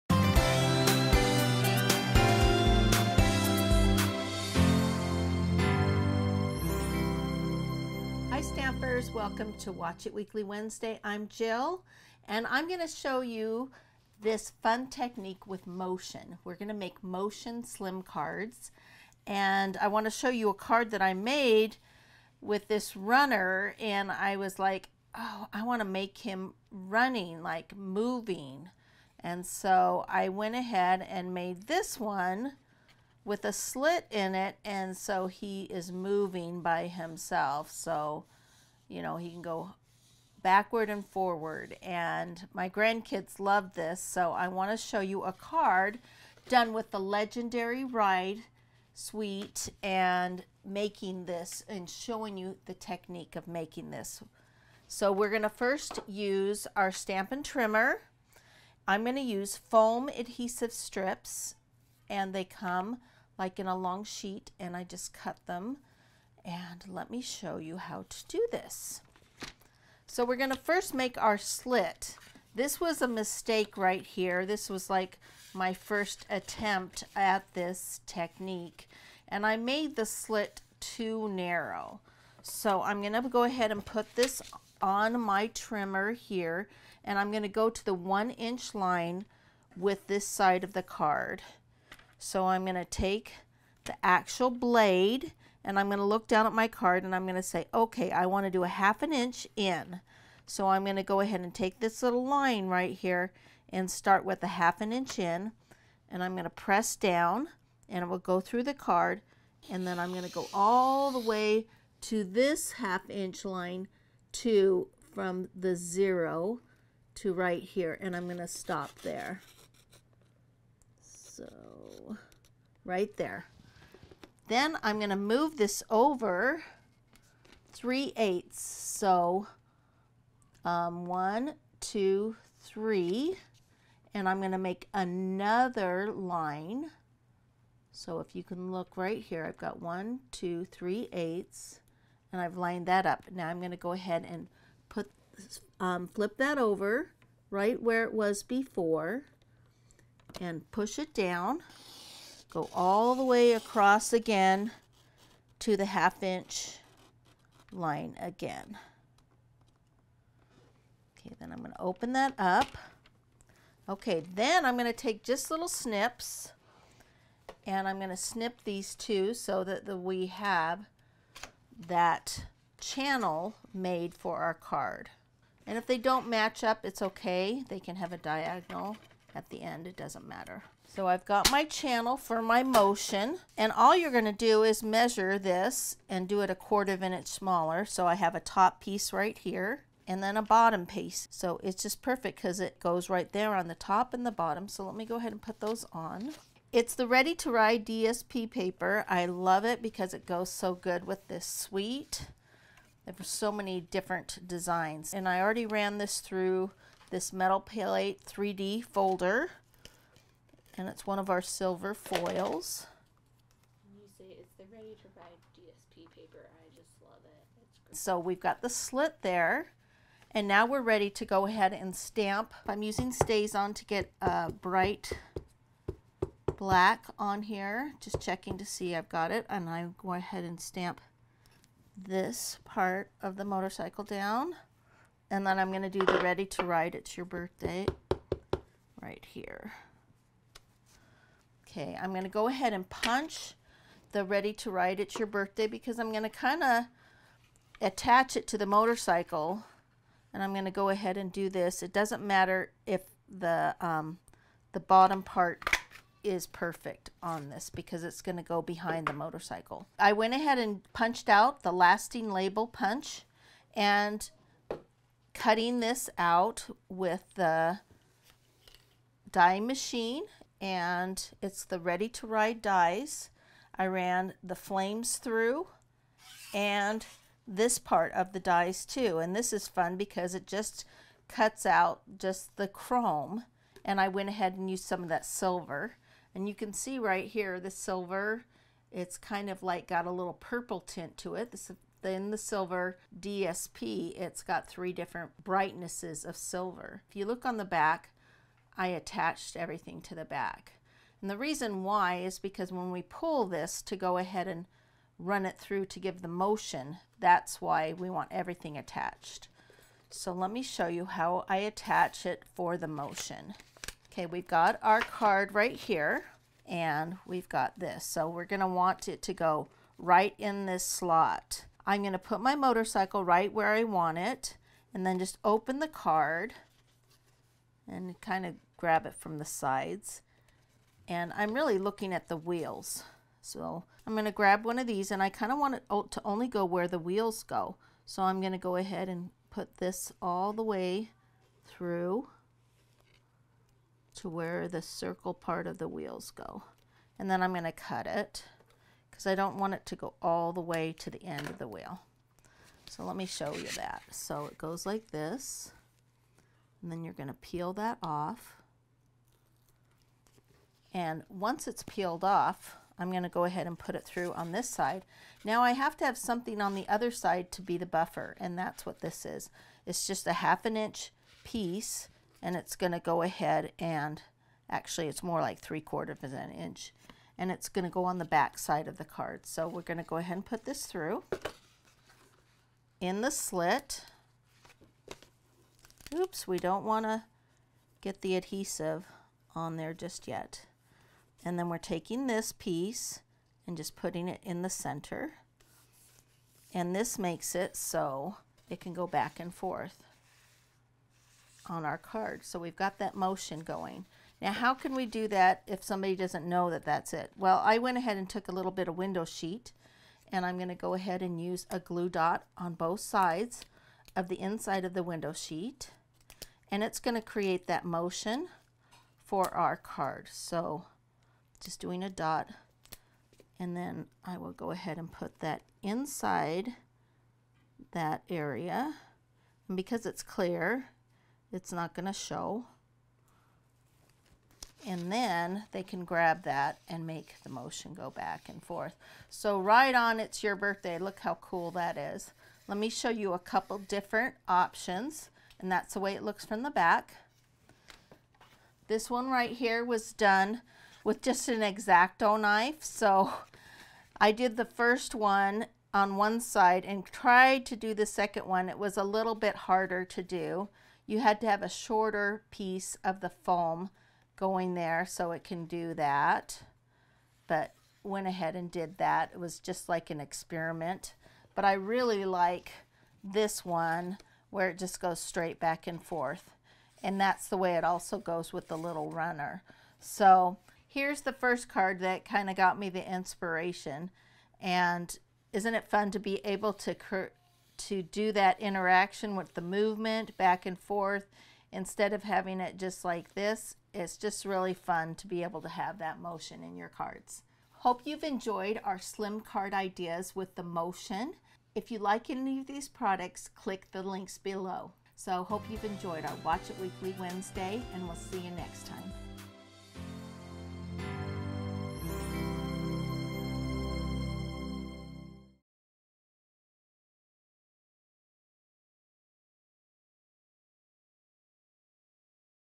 Stamping Jill - Motion Slim Line Card Tutorial
Stampin' Up! Demonstrator Jill Olsen demonstrates a fun way you can create an interactive card. Making a slim card and cutting out a slit or  channel to make a moving object across your card. This idea is a perfect wow card to give someone that's in need. Check out hoe fun and easy this idea can be!

Ready To Ride 12" X 12" (30.5 X 30.5 Cm) Specialty Designer Series Paper [160752]